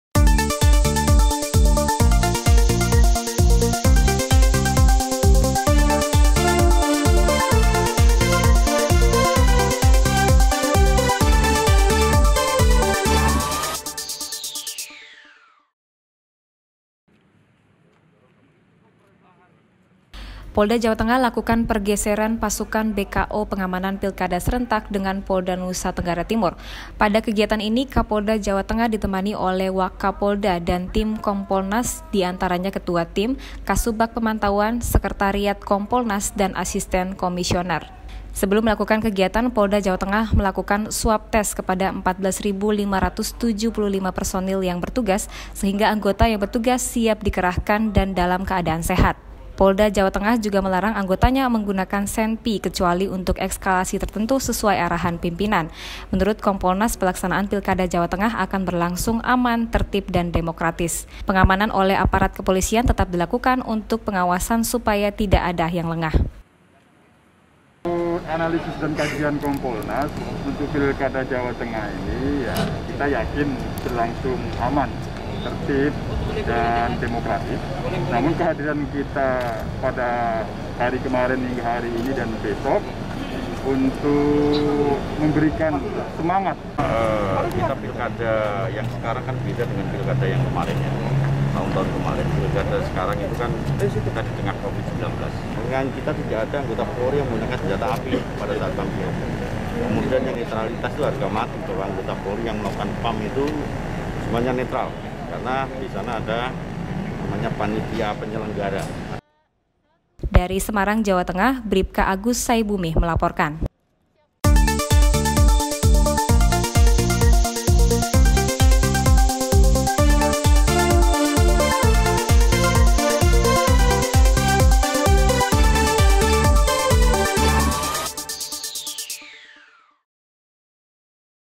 POLDA JATENG LAKSANAKAN PERGESERAN PASUKAN PENGAMANAN PILKADA
Polda Jawa Tengah lakukan pergeseran pasukan BKO pengamanan Pilkada, serentak dengan Polda Nusa Tenggara Timur, pada kegiatan ini Kapolda Jawa Tengah ditemani oleh Wakapolda dan Tim Kompolnas yang diantaranya Ketua TIM, Kasubbag Pemantauan, Sekretariat Kompolnas, dan Asisten Komisioner.

Sebelum melakukan kegiatan, Polda Jawa Tengah lakukan Swab Test kepada 14.575 personil yang bertugas, sehingga anggota yang bertugas siap dikerahkan dan dalam keadaan sehat.

Polda Jawa Tengah juga melarang anggotanya menggunakan Senpi kecuali untuk ekskalasi tertentu sesuai Arahan Pimpinan. Menurut Kompolnas, pelaksanaan Pilkada Jawa Tengah akan berlangsung Aman, Tertib dan Demokratis. Pengamanan oleh Aparat Kepolisian tetap dilakukan untuk pengawasan supaya tidak ada yang lengah.

Tim Liputan : Wahyu Purnama Sari dan Ibnu

 Melaporkan.